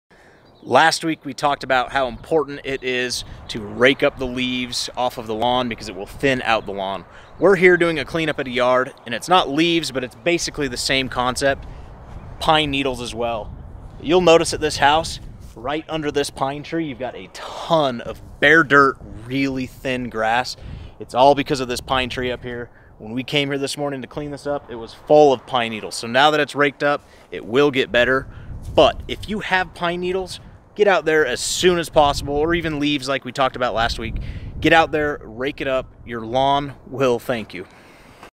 DONT LET LEAVES OR PINE NEEDLES RUIN YOUR LAWN
Make sure to get your lawn nice and tidy for the coming season so their is nothing stopping the healthy growth of your lawn.